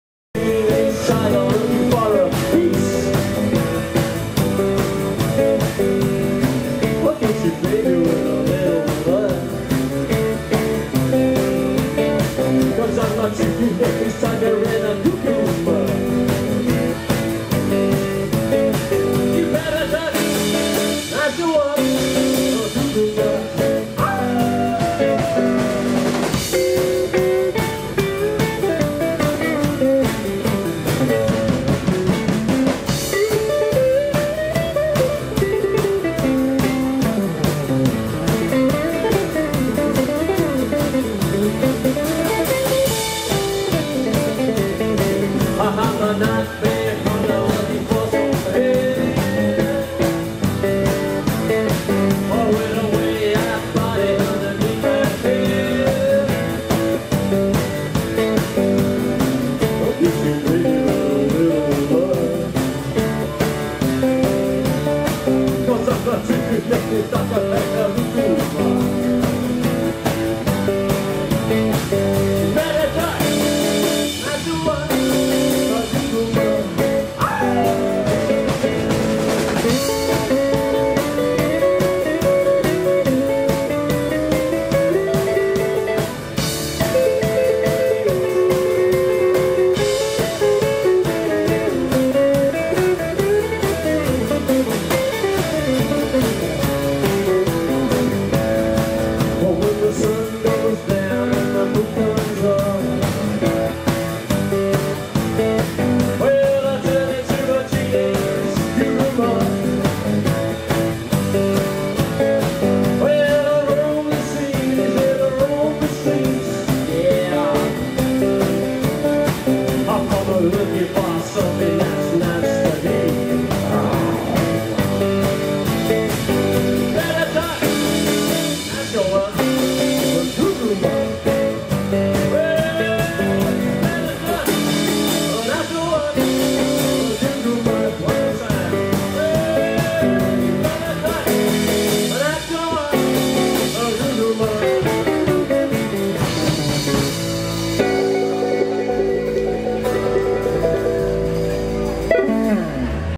The Crawdads playing at the Mustang bar northbridge WA vid23
THE CRAWDADS at the Mustang bar northbridge western australia